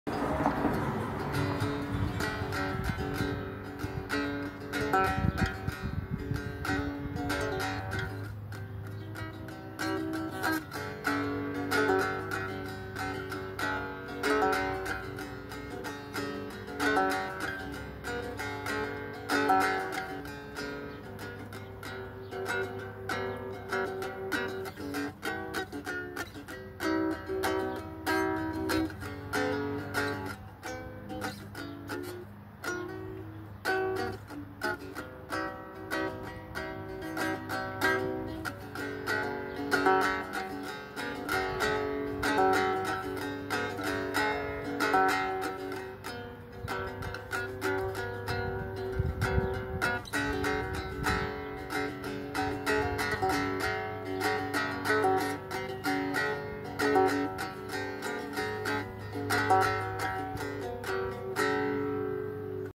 Strumstick
Hand Crafted three-string dulcimer scale Strumstick. These instruments are fun and easy to learn to play. Great for kids and adults alike.